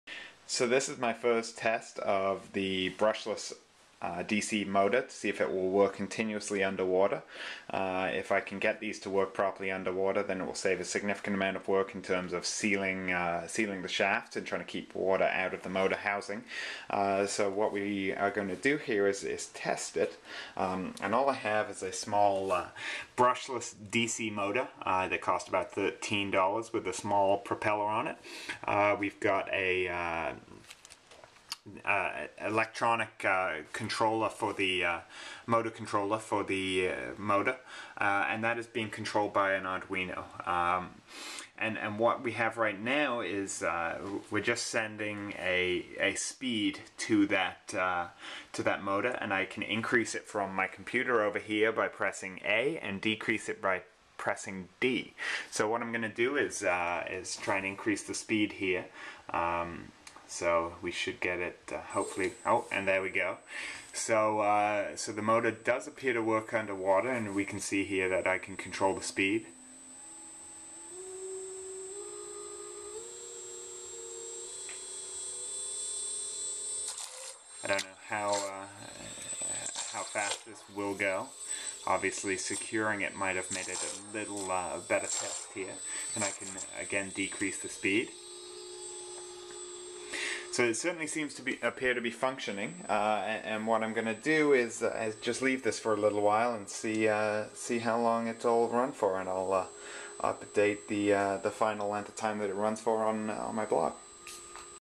Testing Brushless DC Motor Underwater
Testing a Turnigy 2217 Brushless DC Motor underwater for use in an ROV.